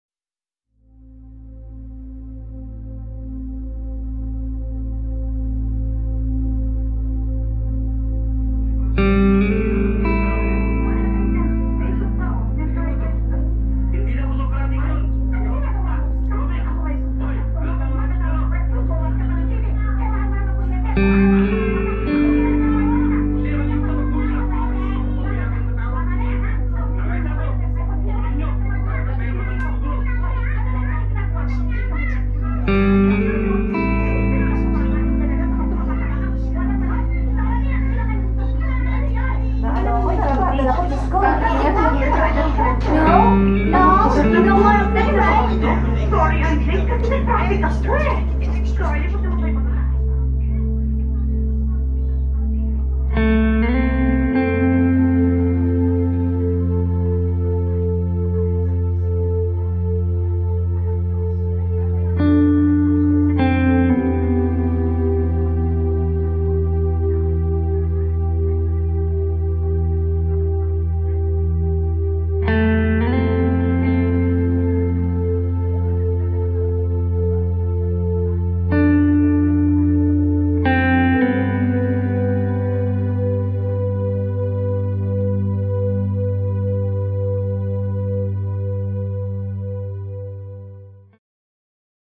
cuz it's unscripted
One summer day when contentions unfold between two cousins.